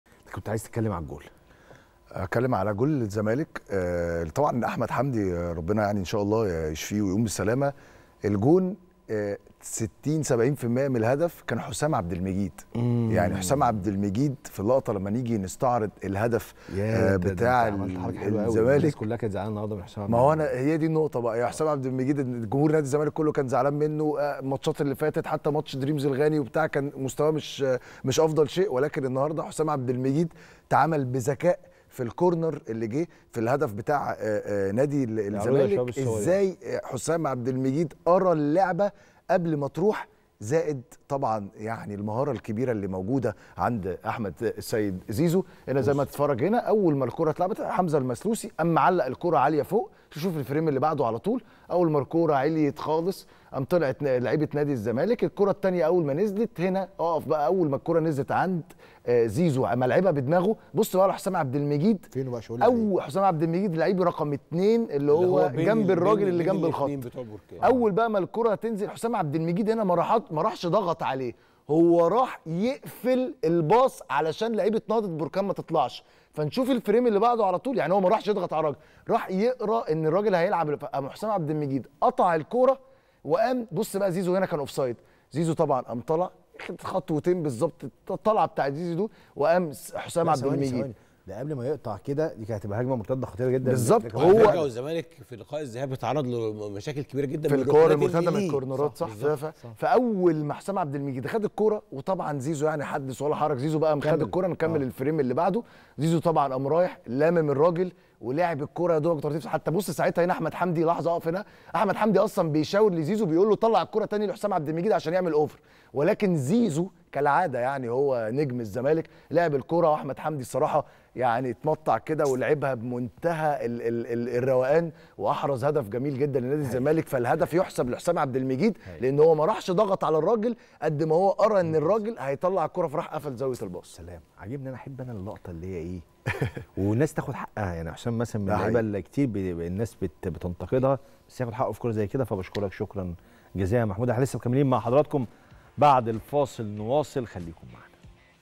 محمود شاكر يستعرض لقطة أول مرة تشوفها لـهدف الزمالك في مرمي نهضة البركان ولا كأنك اول مرة تشوف الهدف
"هدف يحسب لـ حسام عبد المجيد"🚨🚨🚨محمود شاكر يستعرض لقطة أول مرة تشوفها لـهدف الزمالك في مرمي نهضة البركان ولا كأنك اول مرة تشوف الهدف 😱🏹⚪️